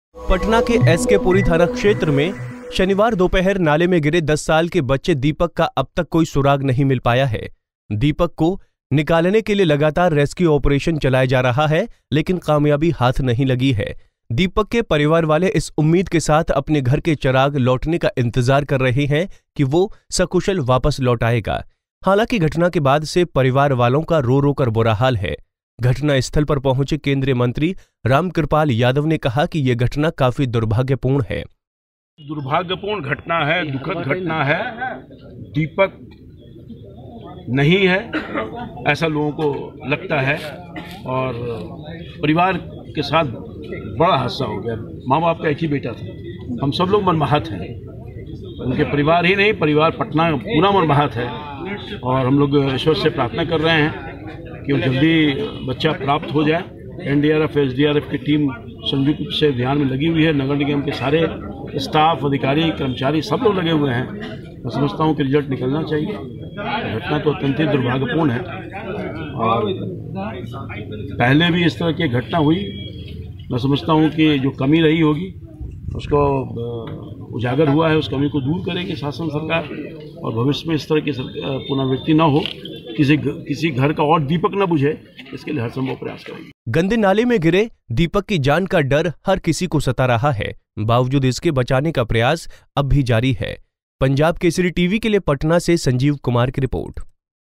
गटर में गिरे दीपक को बचाने के लिए रेस्क्यू ऑपरेशन जारी
गटर में गिरे दीपक को बचाने के लिए रेस्क्यू ऑपरेशन जारी, घटनास्थल पर पहुंचे केंद्रीय मंत्री रामकृपाल यादव